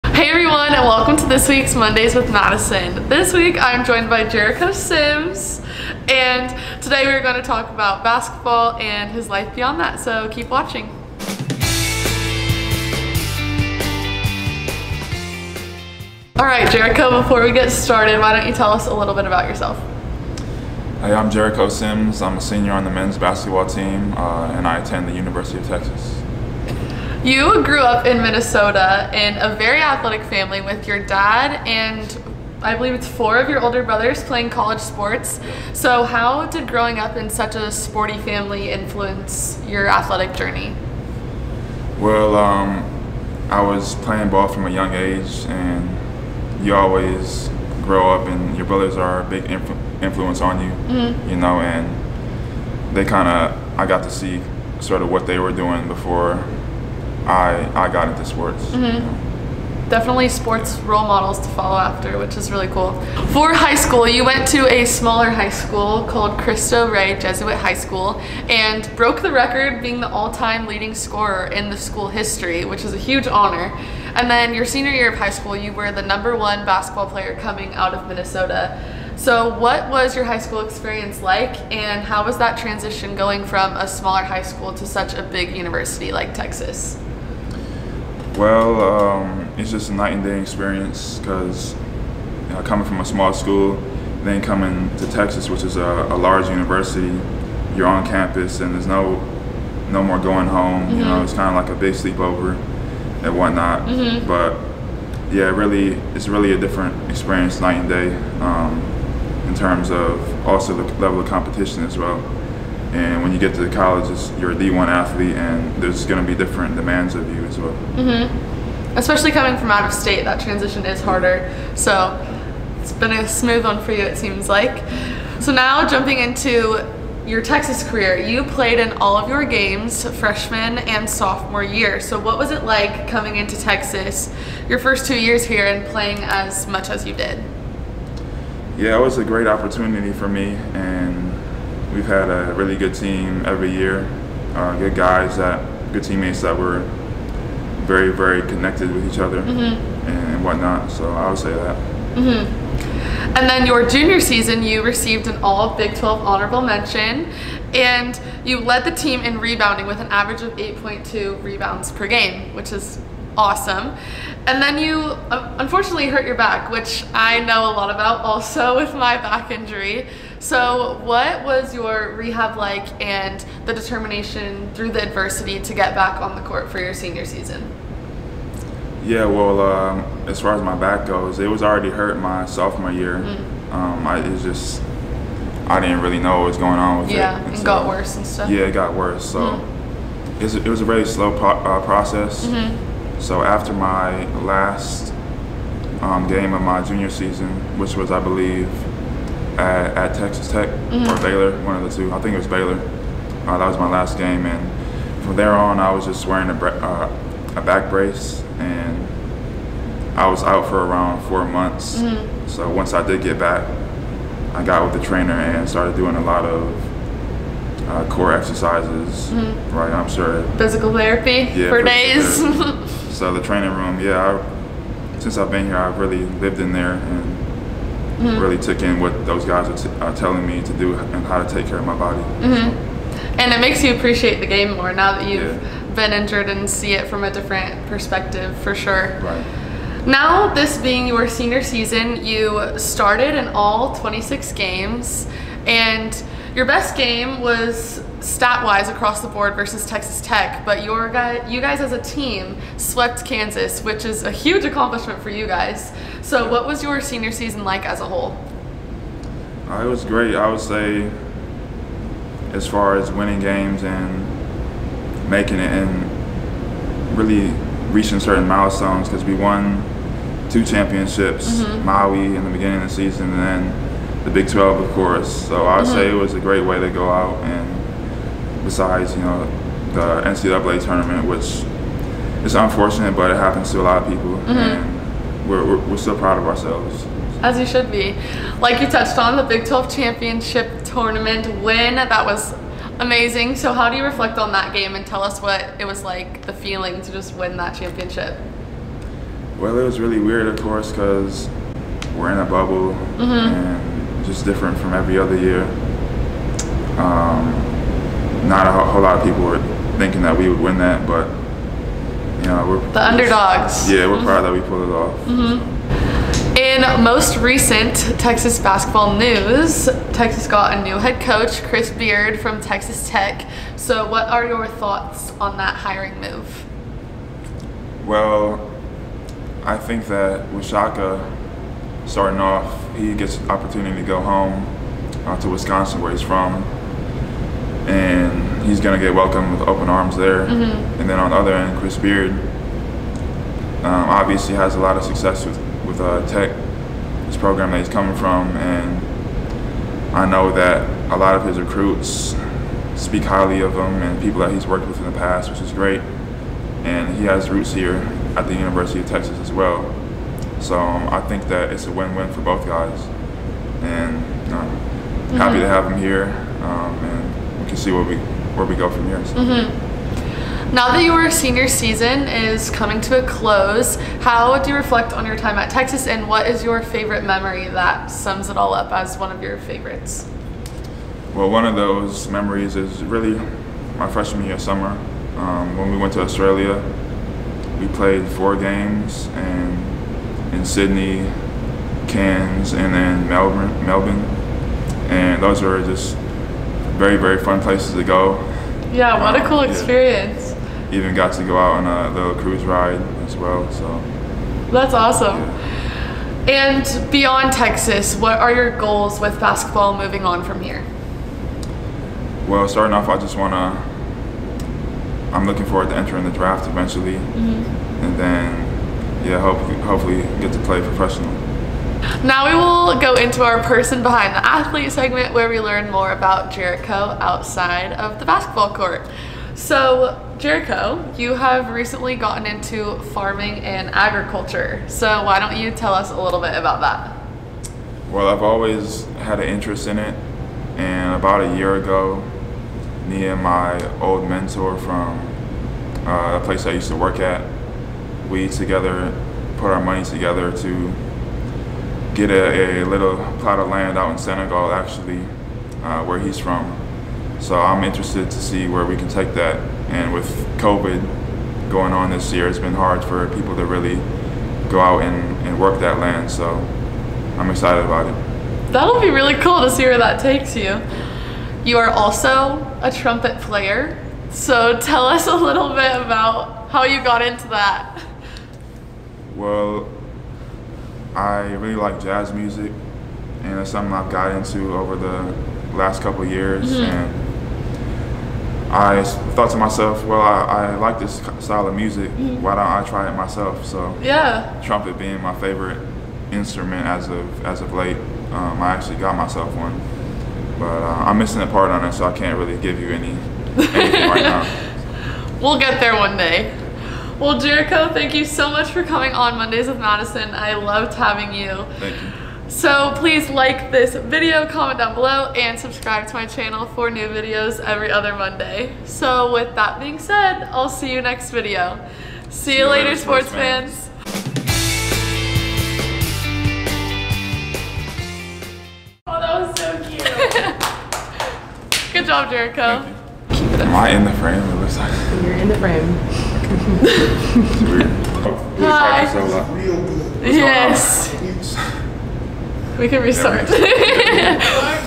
Mondays with Madison ft. Jericho Sims
I had a great time talking to him about basketball, his upbringing, and his interests in agriculture and jazz music. Such a great guy. Hope you all enjoyed. See Ya Later, Sports Fans!

Follow Jericho!